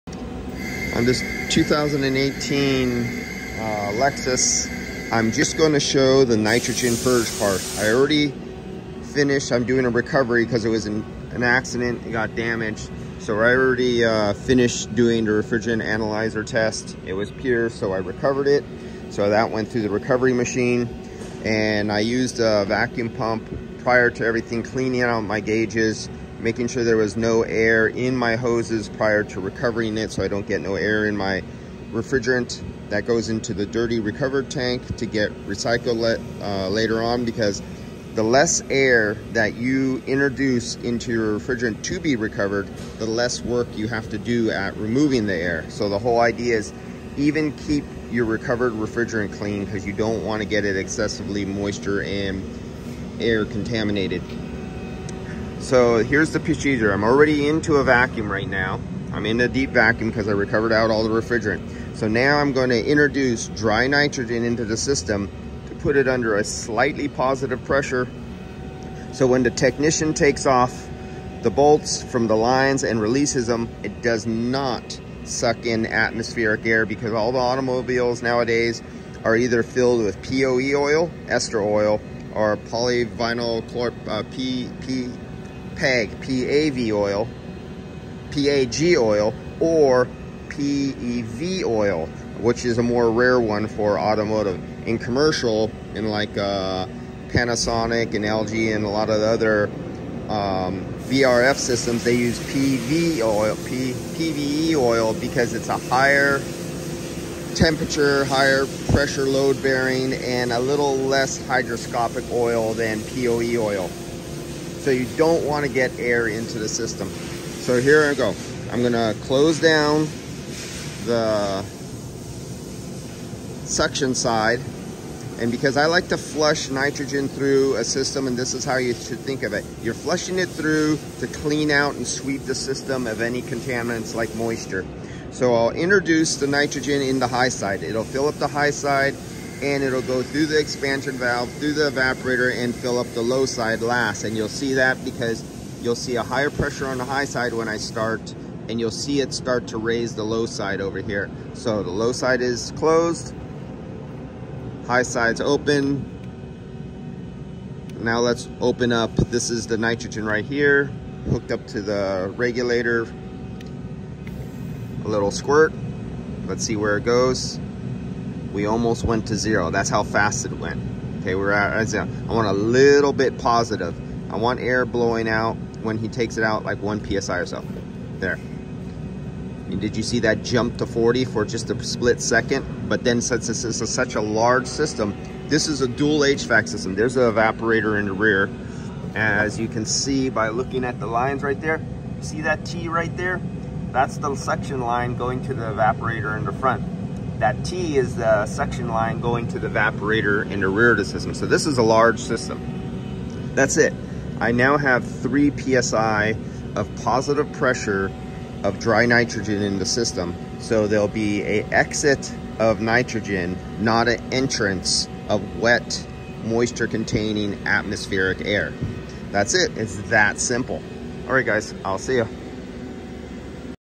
Switching from refrigerant recovery into nitrogen purge to prevent air and moisture contamination￼
DO NOT OPEN A SYSTEM THAT IS UNDER VACUUM!!!￼